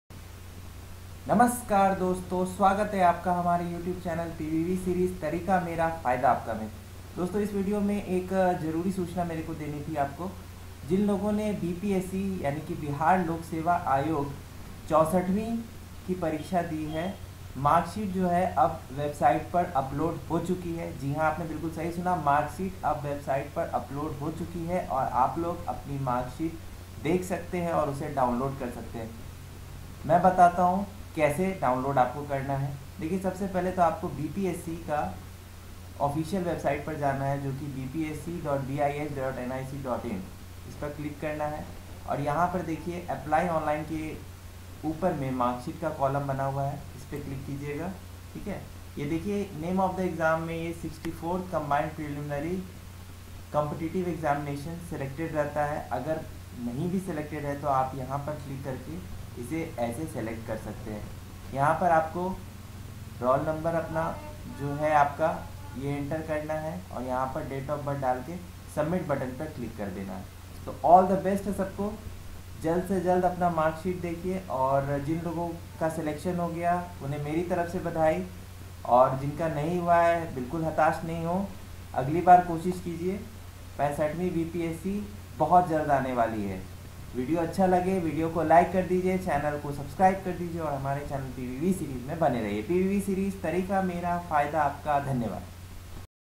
64TH B.P.S.C MARKSHEET ARE OUT NOW
The video explain steps to download the marksheet in pdf format step by step in hindi